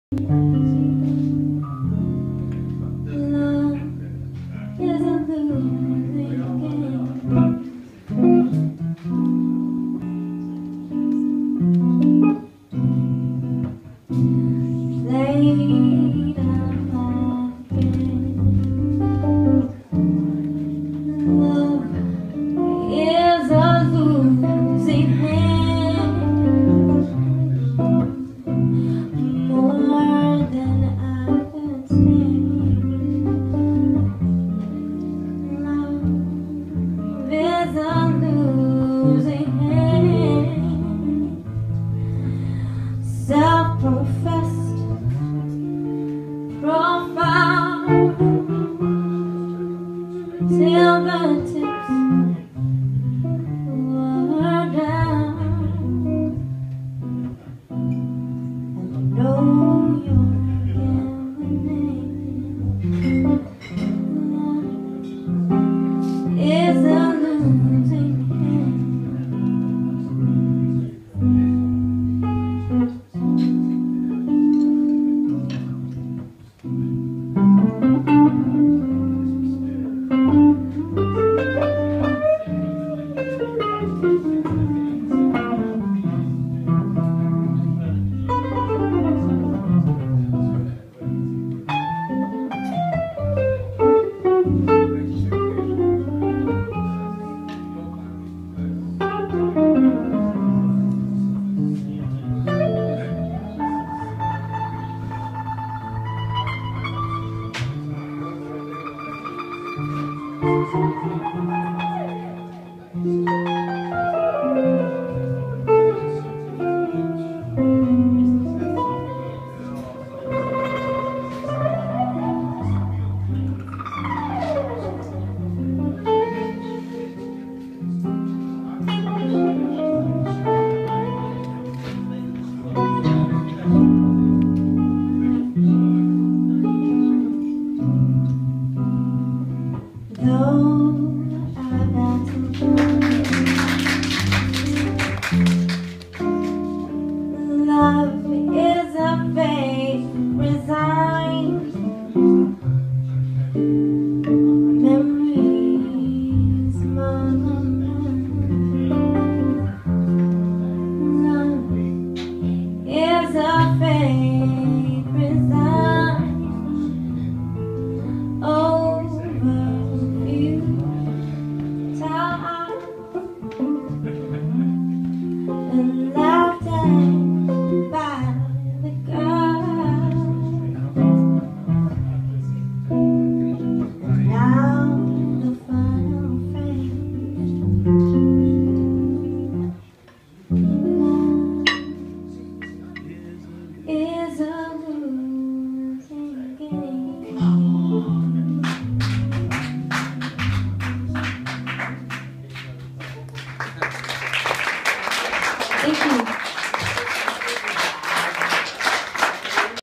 Dasha Logan & Robin Banerjee
Amazing performance at Brasserie Toulouse Lautrec in Kennington in their newly developed 'Loft' Music room.

Dasha is an amazing young singer. Daughter to the famous Malaysian singer Loganathan from the Alleycats.

Robin is a Class A guitarist having performed and toured with Amy Winehouse during a majority of her rise to fame. Personally scouted by Amy Winehouse Robin was ask on numerous occasions to be part of her band. A decision to proud of.